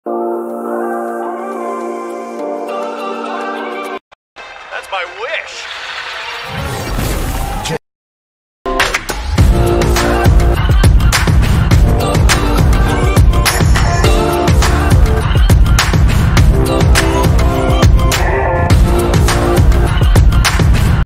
Ninjago NADAKHAN IS BACK! Season 16 Main villain revealed?
Ninjago Creator Drops major Season 16 HINT!

ninjago season 16 official title !